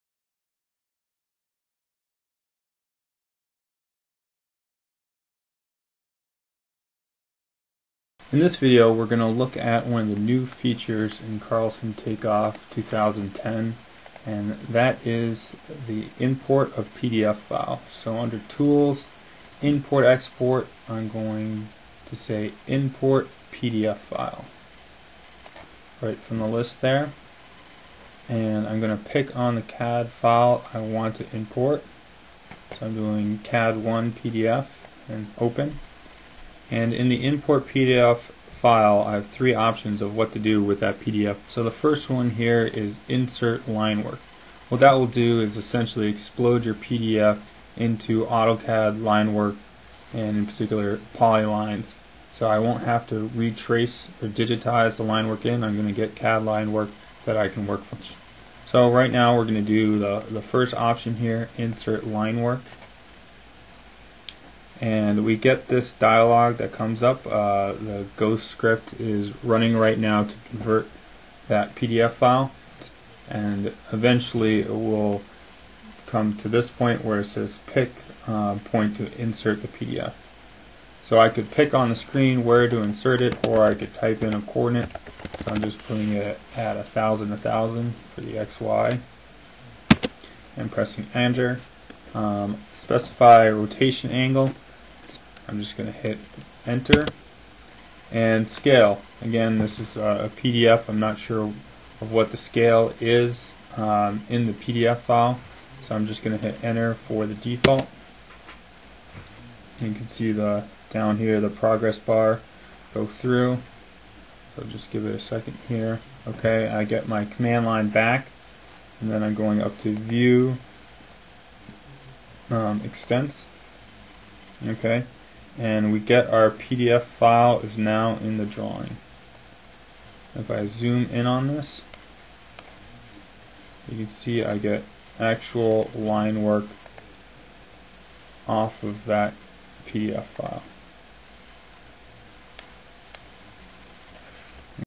PDF Import 1: Getting Started
Selecting and Importing a PDF file into Carlson Takeoff